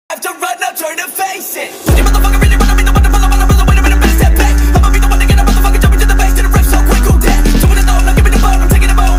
What level am I?
Recently I've been doing some freestyle training and the progress has been really nice so far!